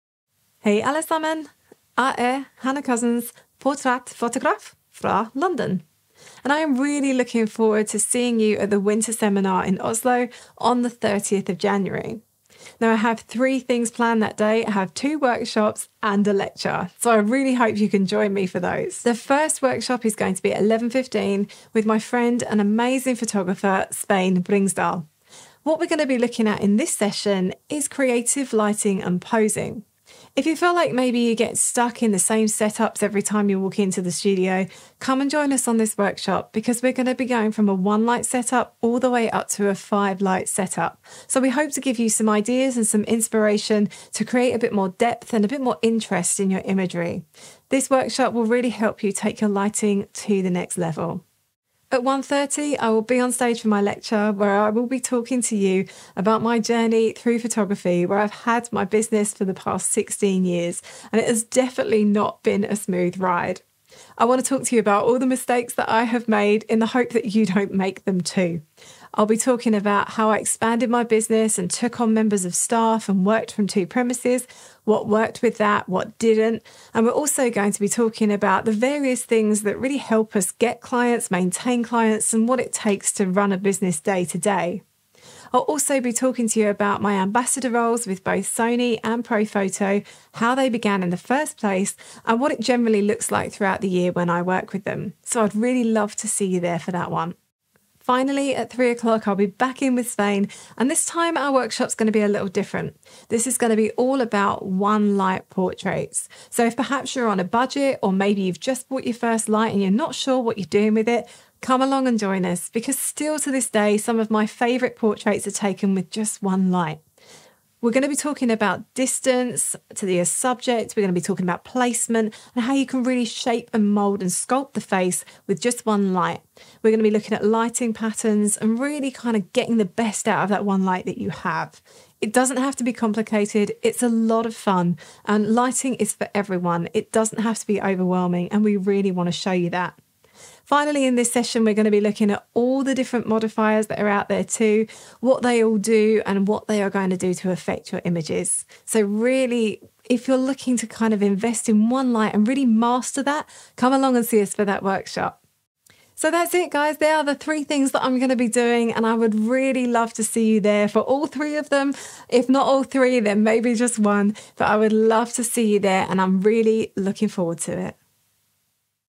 Foredragsholdere - Hannah Couzens
En liten hilsen fra en av våre foredragsholdere på Vinterseminar 2023!